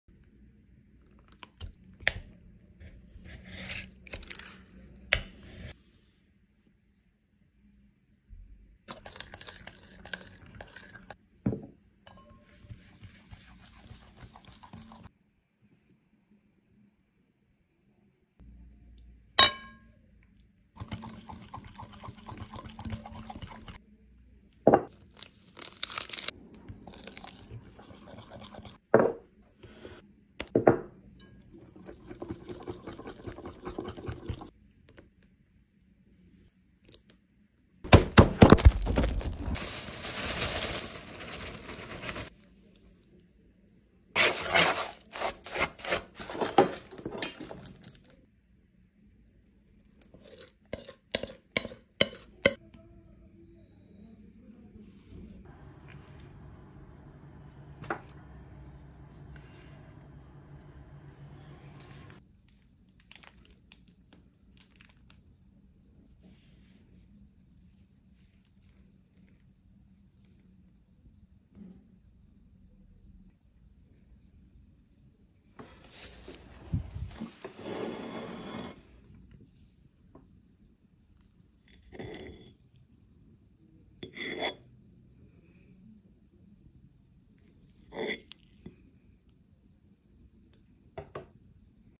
Salepli cheesecake
400 gr labne
1 paket krema
1 bardak salep
3 yemek k un
2 yemek k nişasta
3 yumurta
1 paket vanilin
Yarım su b şeker